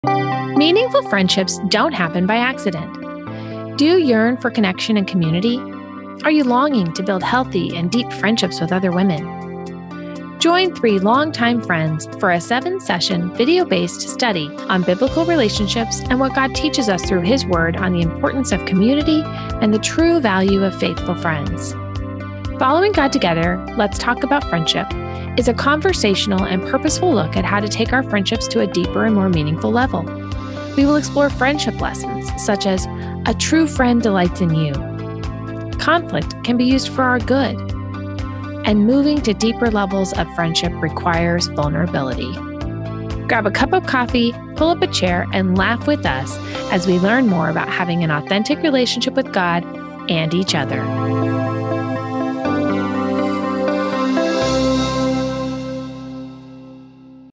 Entwined Ministries Promo Video
Promo video. Entwined Ministries is offering a new Bible study, Following God Together: Let’s Talk About Friendship. Join April, Julie, and Maureen as we study Biblical relationships, share lessons learned from friendships, and realize how to apply those to our own lives.

Promotional Video Filmed By Full Resolution Media